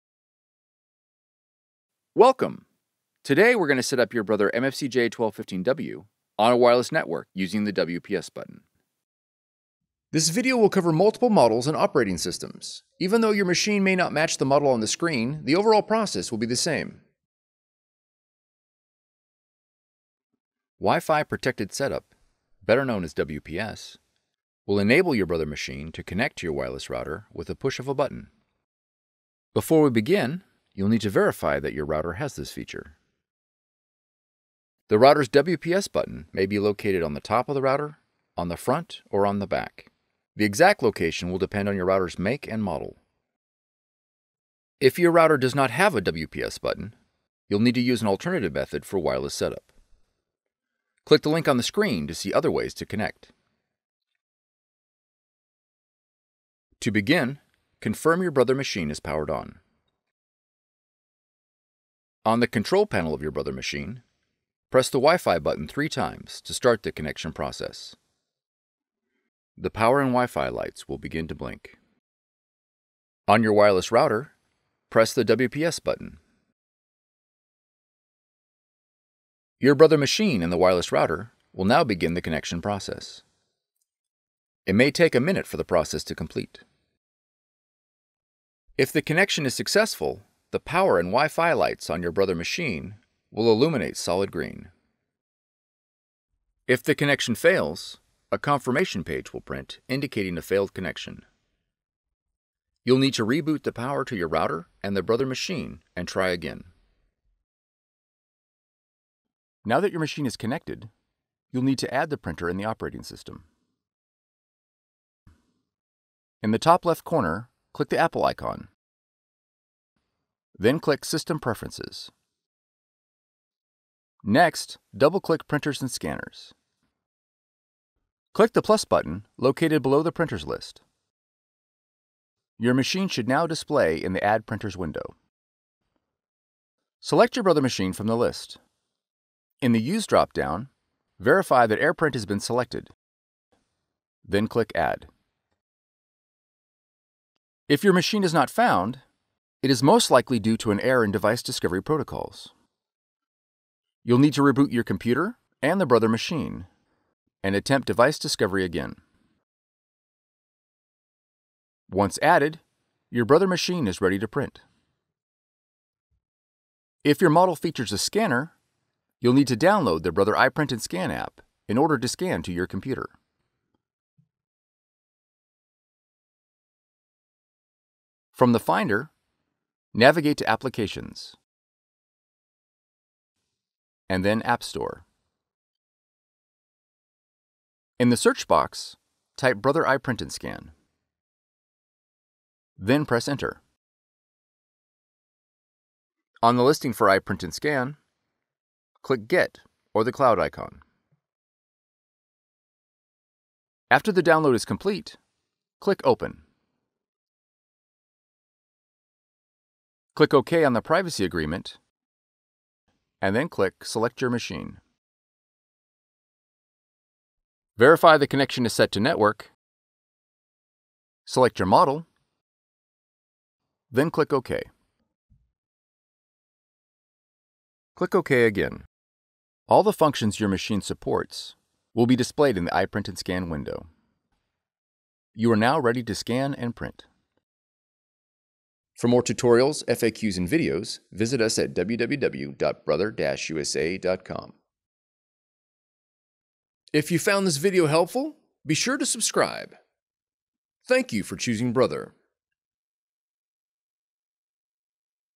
MFCJ1215W set up wireless with WPS - Macintosh
Learn how to set up your Brother MFC-J1215W inkjet printer on a wireless network using Wi-Fi Protected Setup (or WPS) in this video tutorial.

Models covered:
Brother MFCJ1215W

Chapters:
Intro 00:00
Models covered 00:10
Verify router has WPS 00:30
Start process on printer 00:55
Push WPS on router 01:13
Process success or failure 01:30
Add printer in OS - 01:50
AirPrint driver - 02:10
Install iPrint&Scan - 02:45

Transcript:
Welcome, today we are going to setup your Brother MFCJ1215W on a wireless network using the WPS button

Wi-Fi Protected Setup better known as WPS, will enable your Brother machine to connect to your wireless router with a push of a button.

Before we begin, you will need to verify that your wireless router has a WPS Button.

The router’s WPS button may be located on the top of the router, on front or back. The location will depend on your router’s make and model.

If your router does not have a WPS button, you will need to use an alternative methods for wireless setup.  Click the link on the screen to see other ways to connect

To begin, confirm your Brother machine is powered ON.

On your Brother machine, press the Wi-Fi button three times to start the connection process.  The Power and Wi-Fi lights will begin to blink

On the control panel of your wireless router, press the WPS button

Your Brother machine and the wireless router will now begin the connection process.  It may take a minute for the process to complete.

If the connection is successful, the Power and Wi-Fi lights on your Brother machine will illuminate solid green.

If the connection fails, a confirmation page will print indicating a failed connection. You will need to reboot the power to your router and the Brother machine and try again.

Now that your machine is connected, you’ll need to add the printer in the operating system. In the top left corner click the Apple icon. Then click System Preferences. Next, double-click Printers & Scanners

Click the + plus button located below the printers list. Your machine should now display in the Add Printers window.

Select your Brother machine from the list. In the use dropdown verify that AirPrint has been selected. Then click Add.

If your machine is not found it is most likely due to an error in device discovery protocols. You will need to reboot your computer, the Brother machine, and attempt device discovery again.

Once added, your Brother machine is ready to print.

Verify the connection is set to Network. Select your model, then click OK

All the functions your machine supports will be displayed in the  iPrint&Scan window. You are now ready to scan and print

Customers also searched:
Brother MFC J1215W wireless WPS Mac
Brother MFC J1215W wireless WPS setup Mac